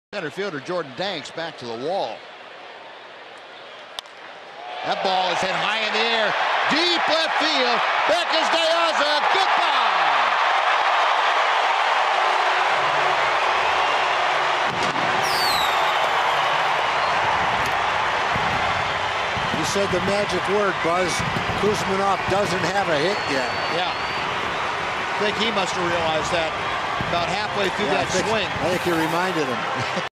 CWS@TEX: Kouzmanoff blasts a solo homer to left field
4/19/14: Kevin Kouzmanoff blasts a solo home run to left field in the 8th inning to extend the Rangers' lead to three runs